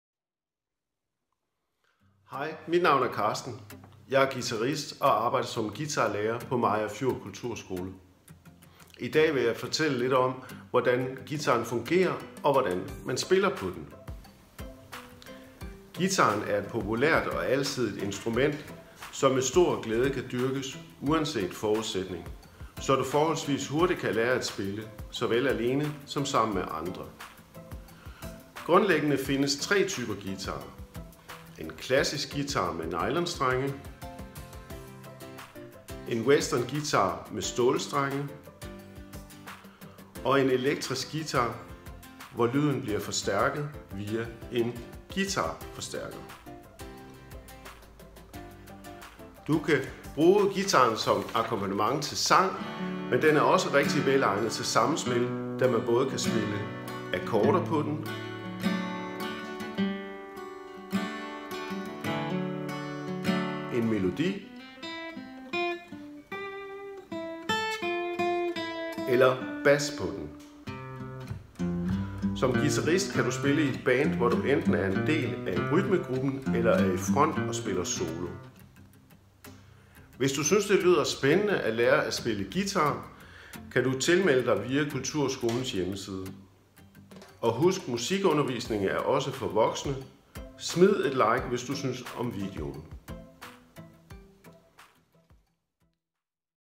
Lær om guitaren
Kulturskolens guitarunderviser Carsten fortæller om hvordan guitaren fungerer, og hvordan man spiller på den.